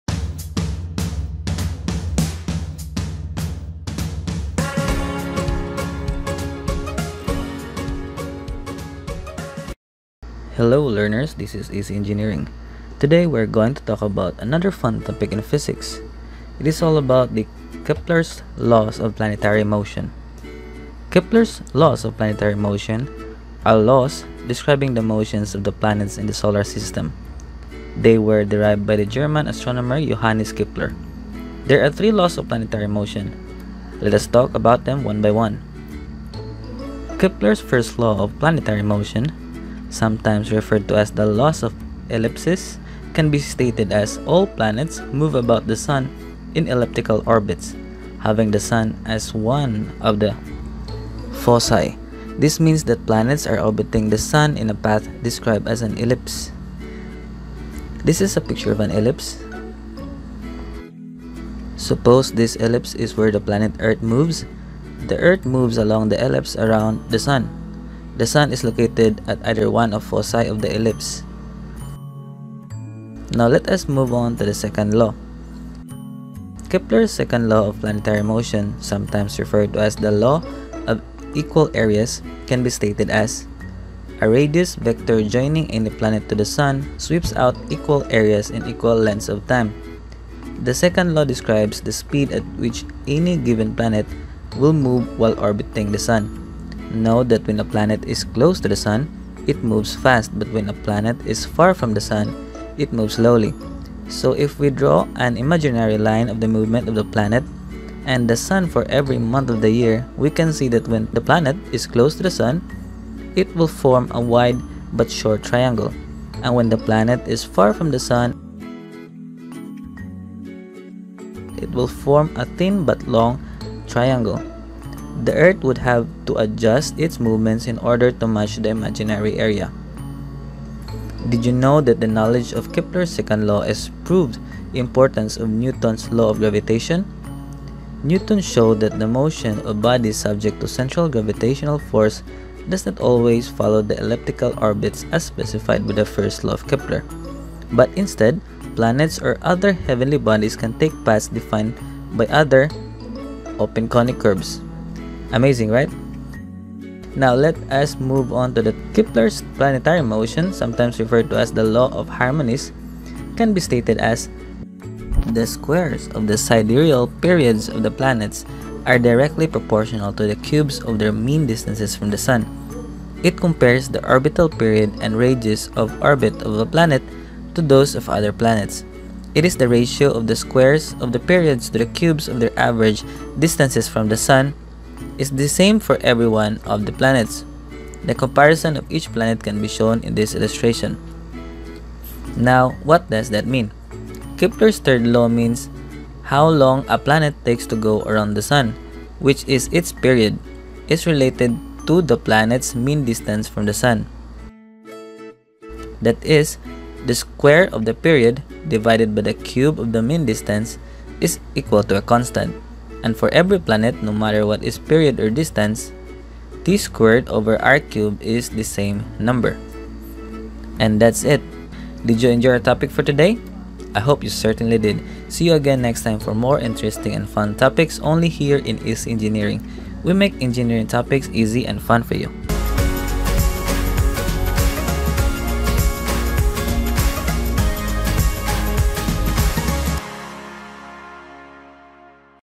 KEPLER'S LAWS | Physics Animation
Today, we are going to talk about Kepler’s Laws of Planetary Motion.

Kepler’s laws of planetary motion are laws describing the motions of the planets in the solar system. They were derived by the German astronomer Johannes Kepler.
There are three laws of planetary motion. Let us talk about them one by one.
Kepler’s first law of planetary motion sometimes referred to as the law of ellipses, can be stated as “All planets move about the Sun in elliptical orbits, having the Sun as one of the foci.” This means that planets are orbiting the sun in a path described as an ellipse.
(Show picture of an ellipse with foci)
This is a picture of an ellipse. Suppose this ellipse is where the planet Earth moves. The Earth moves along the ellipse around the Sun. The Sun is located at either one of the foci of the ellipse.
Now, let us move on to the second law.
Kepler’s second law of planetary motion sometimes referred to as the law of equal areas, can be stated as “A radius vector joining any planet to the Sun sweeps out equal areas in equal lengths of time.”
The second law describes the speed at which any given planet will move while orbiting the sun. Know that when a planet is close to the sun, it moves fast but when a planet is far from the sun, it moves slowly. So if we draw an imaginary line of the movement of the planet and the sun for every month of the year, we can see that when the planet is close to the sun, it will form a wide but short triangle. And when the planet is far from the sun, it will form a thin but long triangle. The earth would have to adjust its movements in order to match the imaginary area.

Find out more by watching the video.

*Enjoy this fun and educational video from EarthPen